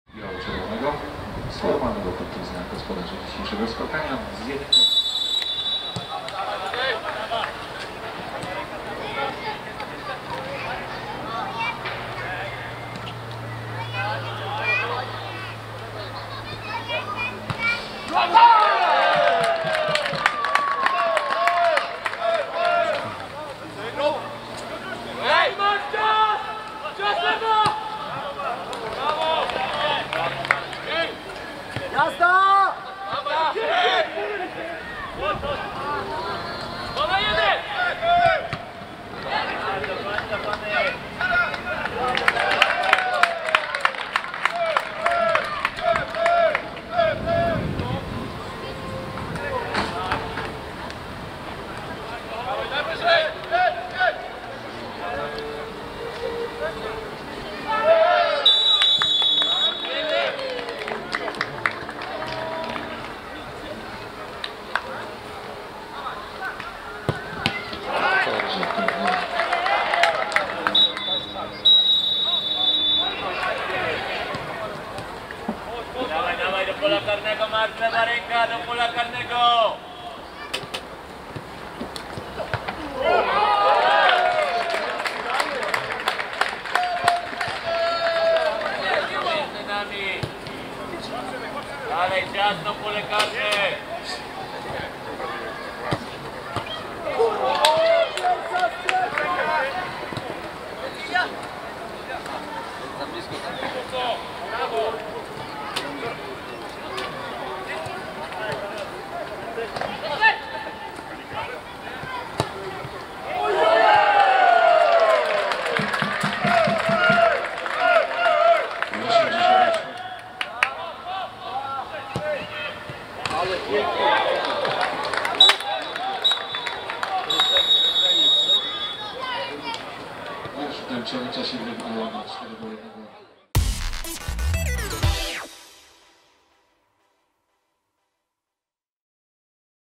LKS Czaniec - Rekord Bielsko-Biała 1:4 (0:2) - Puchar Polski
Ćwierćfinał Pucharu Polski na szczeblu Podokręgu i pewna wygrana biało-zielonych.